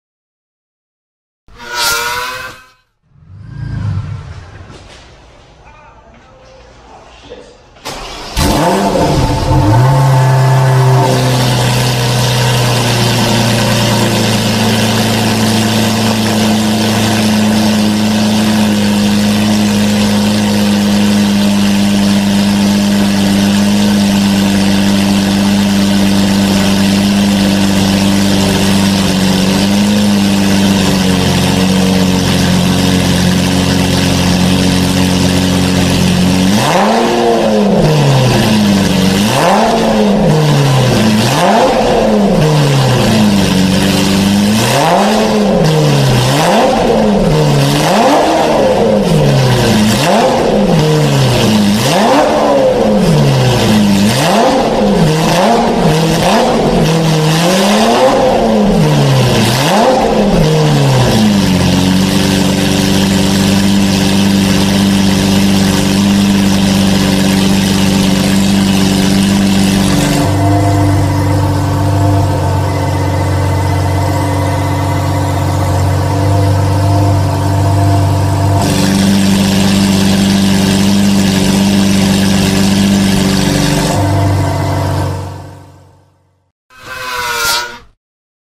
Lamborghini Murcielago LP640 installed Fi Exhaust Amazing Loud Revs !!!
Video Fitted with FI Complete Exhaust System includes:
Ultra High Flow Catless Pipe (Valvetronic)
Muffler

for more product information.